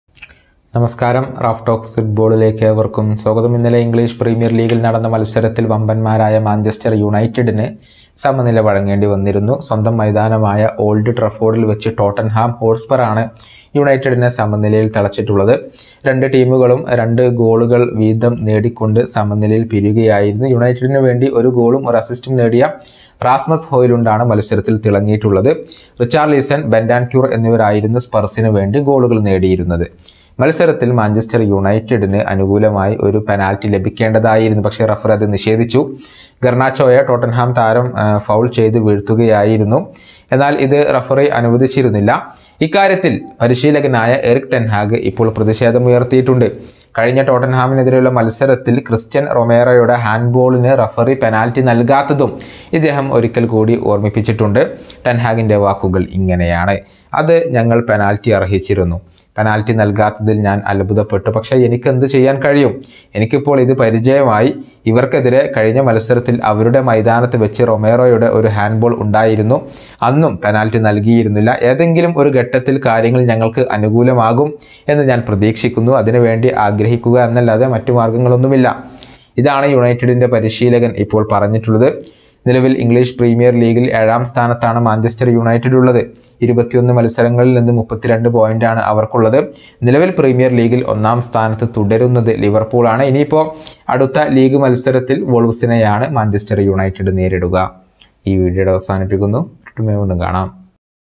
അന്ന് റൊമേറോയുടെ ഹാൻഡ് ബോൾ,ഇന്നും അതിന് മാറ്റമില്ല: പെനാൽറ്റി കിട്ടാത്തതിൽ പ്രതിഷേധിച്ച് ടെൻ ഹാഗ്!
Manchester United vs Tottenham
Ten Hag